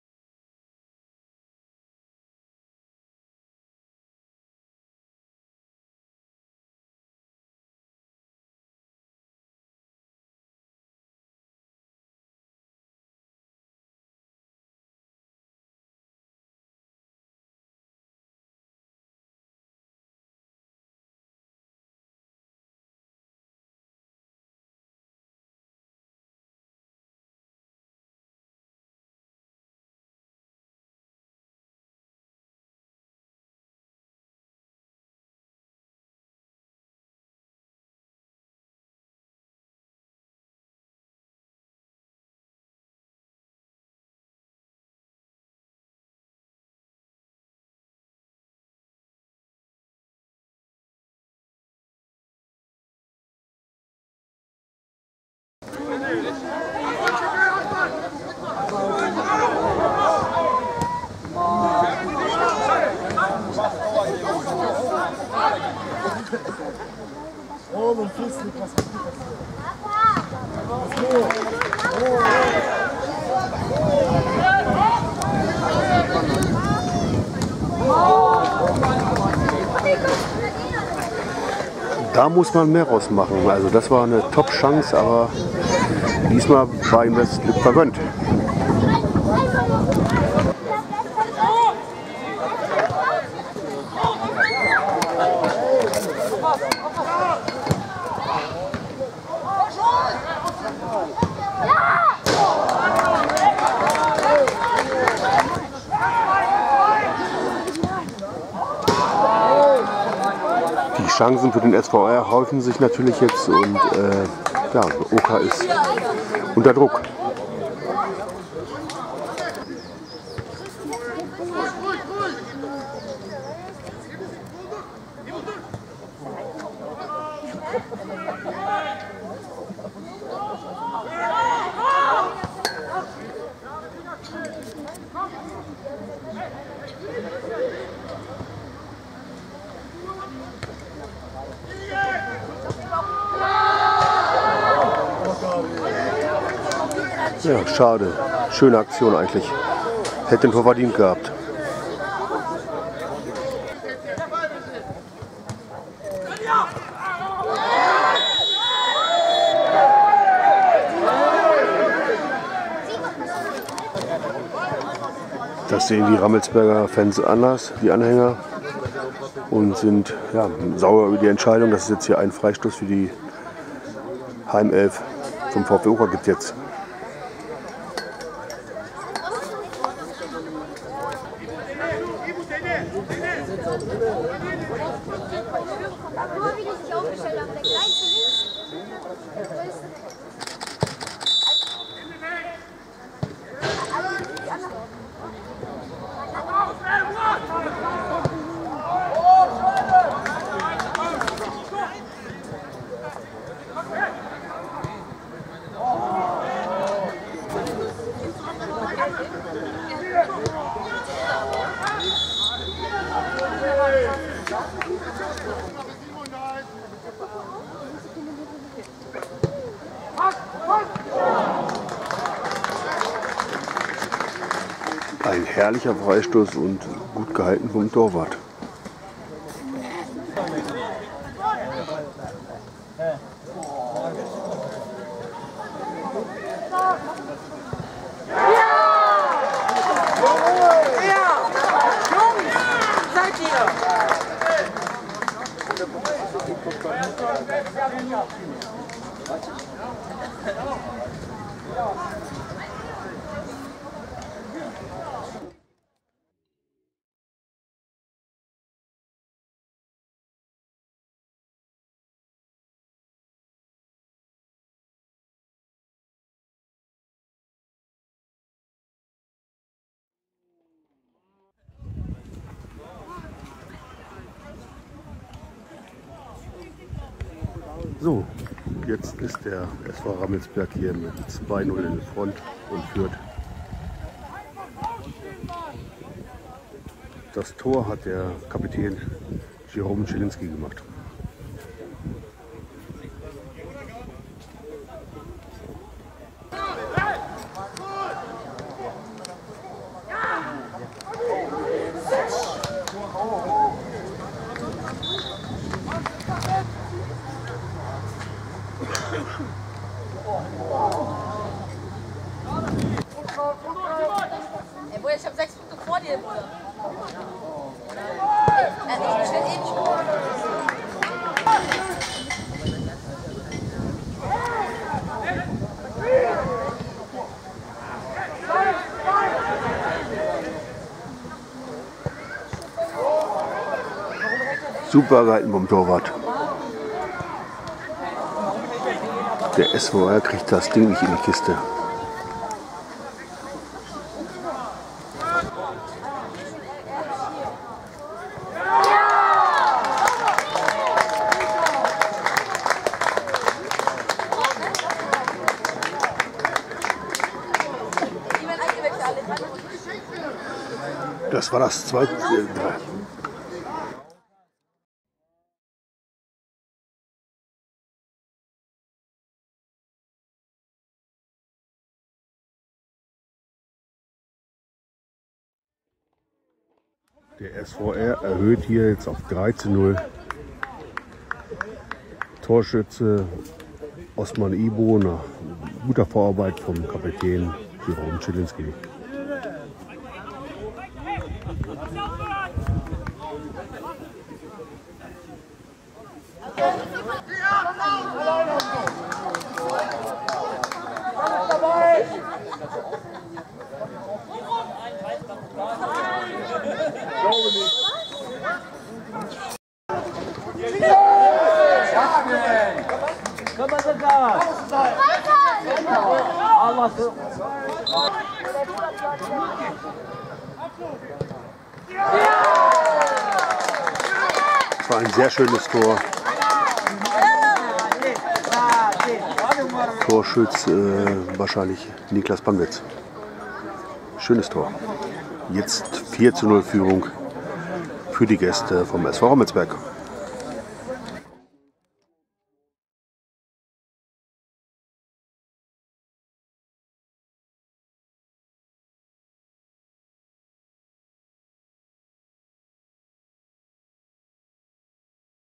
Fussball-Harz TV, 25.08.2024, VfL Oker II - SV Rammelsberg 0:4 (0:1), Nordharzliga 1, 3. Spieltag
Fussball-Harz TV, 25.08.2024
Nordharzliga 1, 3. Spieltag
VfL Oker II - SV Rammelsberg 0:4 (0:1)
Tore:
0:1 (20.) Ibo
0:2 (55.) Schliesenski
0:3 (72.) Ibo
0:4 (78.)Pannwitz